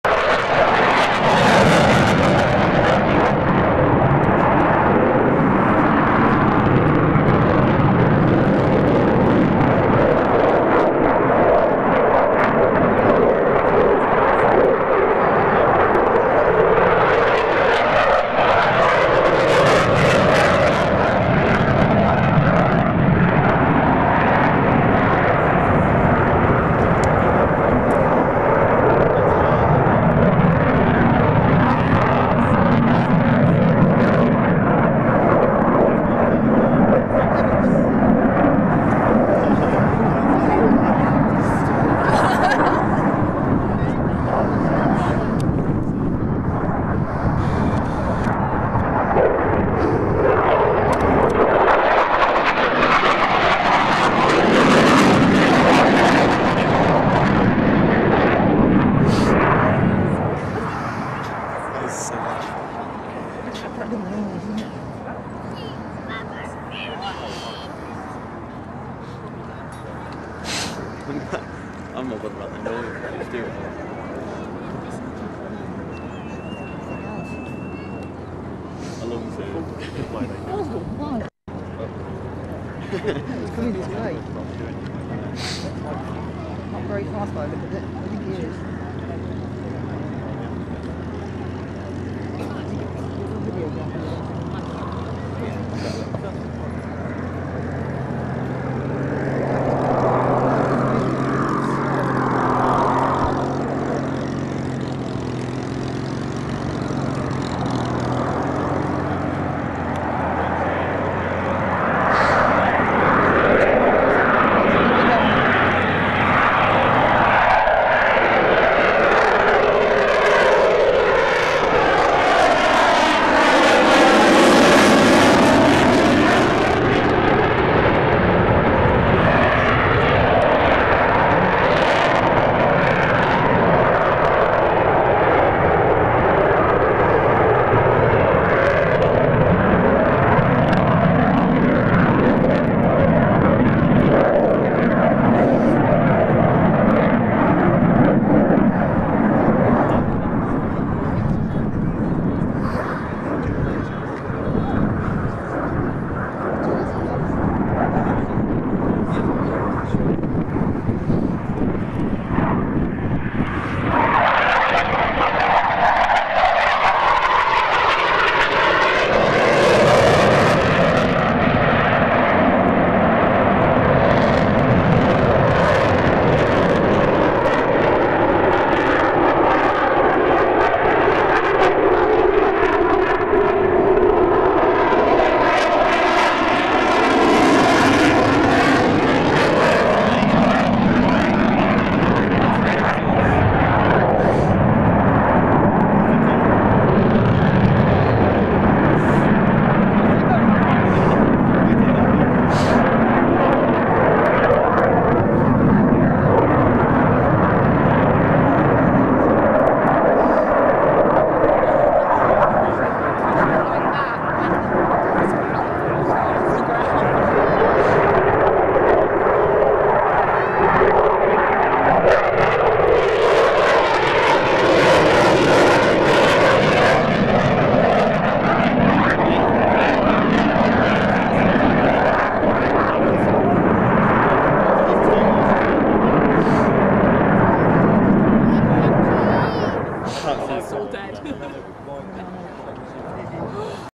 EF Typhoon solo
Battle of Britain Memorial Flight  50th Anniversary, Duxford, England. 5/5/07.